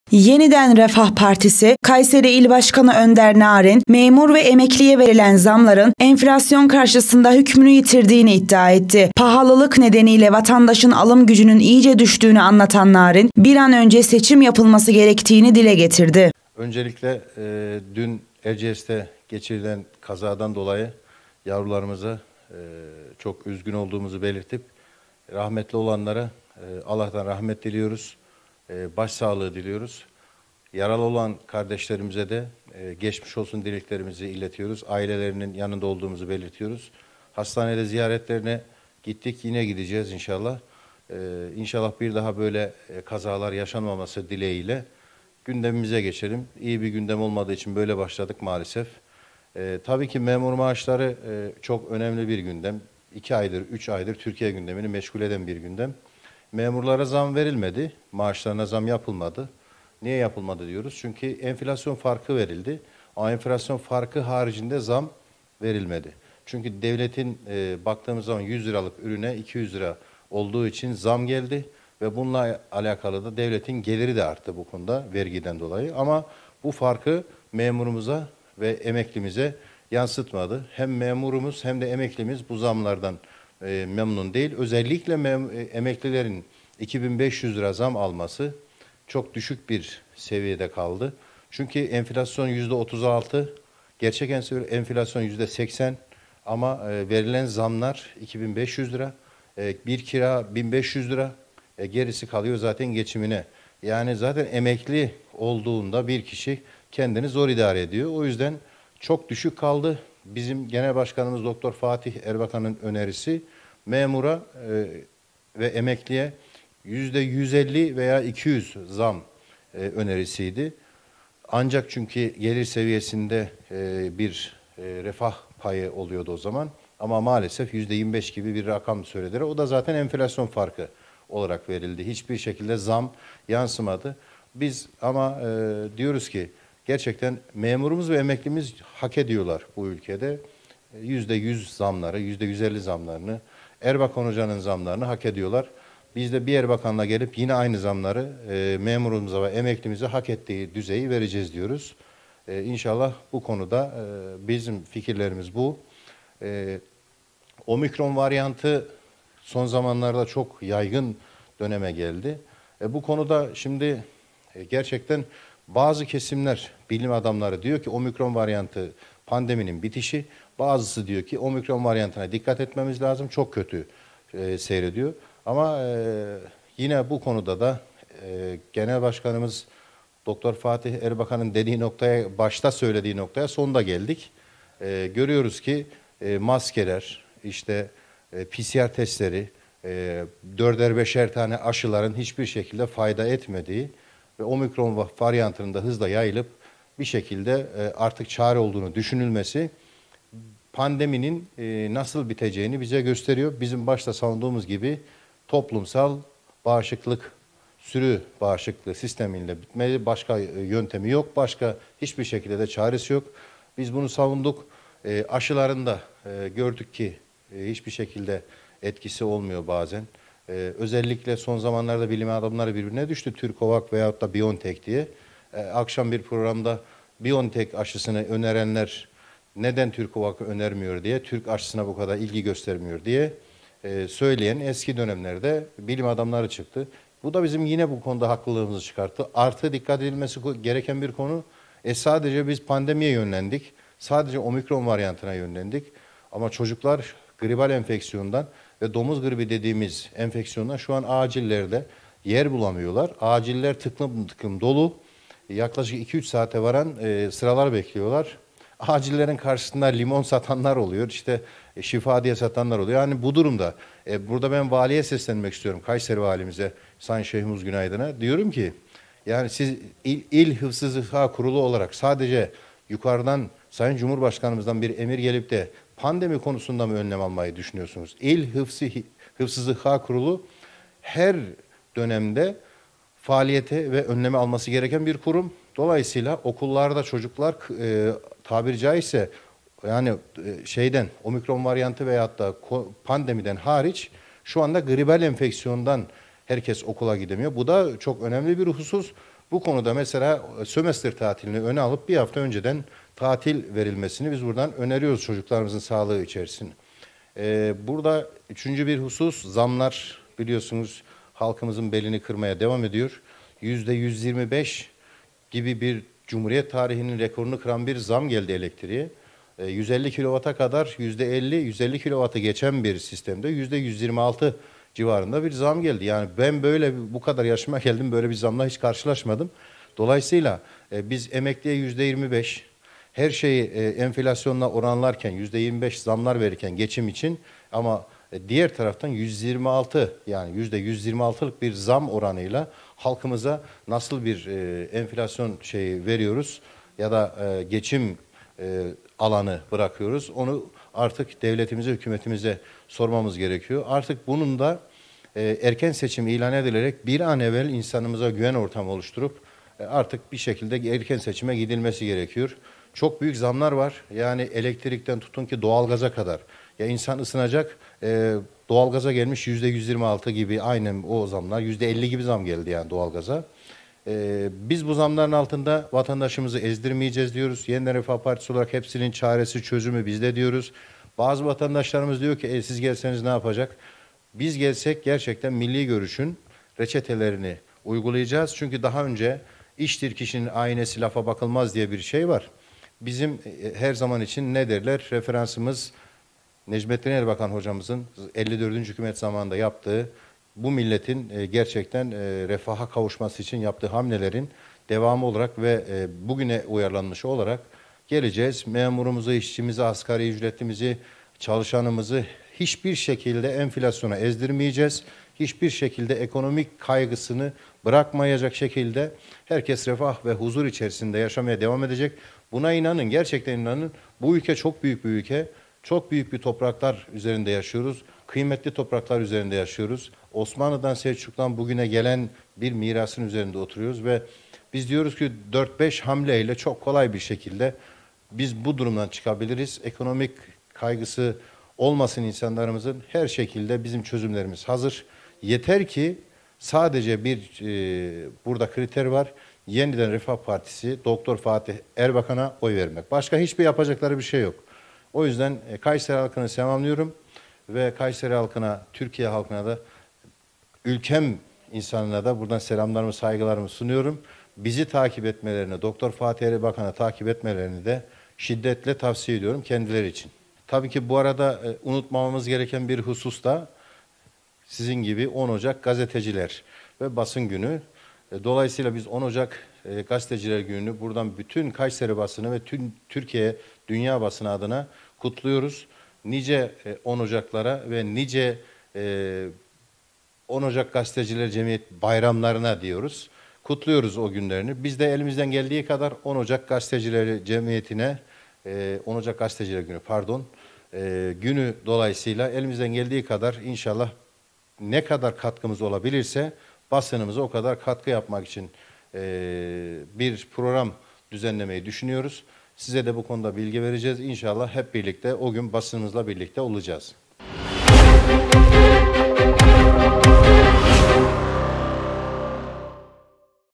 YRP Başkanı Narin: “Memura verilen maaş artışı, enflasyon karşısında kayboldu”
KAYSERİ, (BGC – ÖZEL HABER) - Yeniden Refah Partisi (YRP) Kayseri İl Başkanı Önder Narin, memur ve emekliye verilen zamların enflasyon karşısında hükmünü yitirdiğini iddia etti. Pahalılık nedeniyle vatandaşın alım gücünün iyice düştüğünü anlatan Narin, bir an önce seçim yapılması gerektiğini dile getirdi.

YRP Kayseri İl Başkanı Narin, KBGC mikrofonlarına gündemi değerlendirdi. Erciyes dönüşü yaşanan kazada hayatını kaybedenlere başsağlığı dileyen Narin, yaralılara da acil şifalar temennisinde bulundu. Memur ve emekli maaşlarının son günlerin birinci gündem konusu olduğunu ifade eden Narin, maaş artışlarının enflasyon karşısında çok düşük kaldığını öne sürdü. “Memurların maaşlarına zam yapılmadı” diyen Narin, “Niye yapılmadı diyoruz? Çünkü enflasyon farkı verildi. Çünkü 100 liralık ürün 200 lira oldu. Bununla alakalı vergiden dolayı devletin geliri de arttı Hem memurumuz hem de emeklimiz bu zamlardan memnun değil. En düşük emekli maaşının 2 bin 500 lira olması çok da düşük bir oran. Çünkü enflasyon yüzde 36, gerçek enflasyon yüzde 80.  kira bin 500 lira. Gerisi kalıyor tabii ki geçimine. Yani zaten bir kişi emekli olduğunda kendini zor idare ediyor. O yüzden çok düşük kaldı. Genel başkanımız Fatih Erbakan’ın önerisi memura ve emekliye yüzde 150, yüzde 200 zamdı. Ancak gelir seviyesinde bir refah payı oluyordu o zaman. Maalesef yüzde 25 bir rakam söylediler. Bu da zaten enflasyon farkı olarak verildi, hiçbir şekilde zam yansımadı” dedi.

“ZAMLAR VATANDAŞIN BELİNİ BÜKTÜ”

Ürünlere gelen zamların halkın belinibüktüğünü anlatan YRP Kayseri İl Başkanı Narin, elektrik ve doğalgaz kullanımana gelen yüksek orandaki zamlara dikkat çekti. Elektriğe cumhuriyet tarihinin rekor zamlarından birisini yapıldığını öne süren Narin, “Elektriğe yüzde 125 gibi cumhuriyet tarihinin rekorunu kıran bir zam geldi. 150 kilowatta kadar yüzde 50, 150 kilowatı geçen abonelere ise yüzde 126 civarında bir zam. Ben bu yaşıma kadar böyle bir zamla karşılaşmadım. Dolayısıyla biz emekliye yüzde 25, her şeyi enflasyona oranlarken yüzde 25 zamlar verirken geçim için ama diğer taraftan yüzde 126 zam oranıyla halkımıza nasıl bir enflasyon farkı veriyoruz ya da geçim alanı bırakıyoruz? Bunu devletimize, hükümetimize sormamız gerekiyor. İnsanlar ısınacak doğalgaza gelmiş yüzde 50 kadar bir zam” diye konuştu.

“İKTİDARIMIZDA ÇALIŞANLARI ENFLASYONA EZDİRMEYECEĞİZ”

Biz bu zamlar altında vatandaşımızı ezdirmeyeceğiz. Yeniden Refah Partisi olarak hepsinin çözümü bizde diyoruz. Bazı vatandaşlarımız diyor ki ‘Siz gelseniz ne yapacaksınız?’ Biz gelsek, gerçekten milli görüşün reçetelerini uygulayacağız. Çünkü daha önce iştir kişinin aynası lafa bakılmaz diye bir şey var. Bizim için her zaman ne derler referansımız Necmettin Erbakan hocamızın 54’üncü hükümet zamanında milletin gerçekten refaha kavuşması için yaptığı hamlelerin devamı olarak bugüne uyarlanmışı olarak geleceğiz. Memurumuzu, işçimizi, emeklimizi hiçbir şekilde enflasyona ezdirmeyeceğiz. Hiçbir şekilde ekonomik kaygısı bırakmayacağız. Herkes refah ve huzurlu bir şekilde yaşamaya devam edecek.  Buna gerçekten inanın. Bu ülke çok büyük bir ülke, çok büyük topraklar üzerinde yaşıyoruz, kıymetli topraklar üzerinde yaşıyoruz. Osmanlı2dan Selçuklulardan bugüne kadar gelen bir mirasın üzerinde yaşıyoruz. Biz diyoruz ki 4-5 hamleyle çok kolay bir şekilde biz bu durumdan çıkabiliriz. İnsanlarımızın ekonomik kaygısı olmasın, her şekilde bizim çözümlerimiz hazır. Yeter ki Yeniden Refah Partisi, Fatih Erbakan’a oy vermek” dedi.